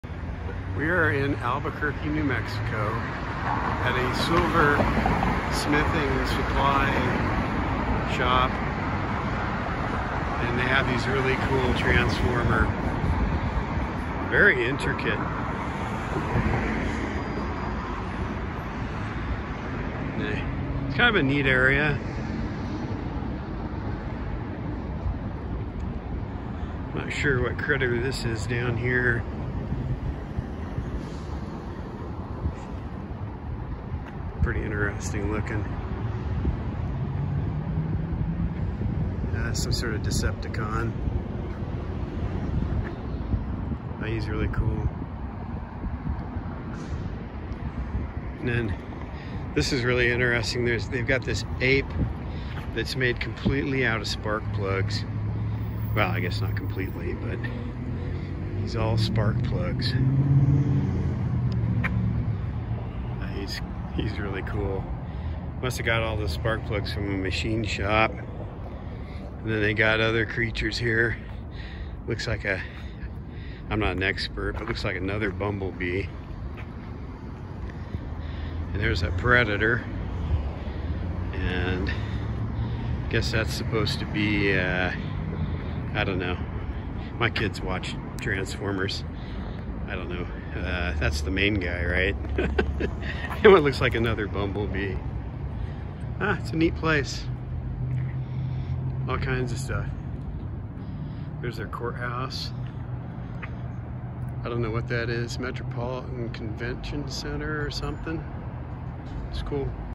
Interesting statues including Transformers at a silver shop in down town Albuquerque
We stumbled on this interesting display while near the rail Yards in Albuquerque.  Primarily Transformers but there was this cool ape made up mostly of spark plugs.  Inside there was a life size Iron Man figure and lots of smaller figures as well.  They all had price tags on them and they were quite expensive.  They don't allow filming inside but it was a neat place to visit.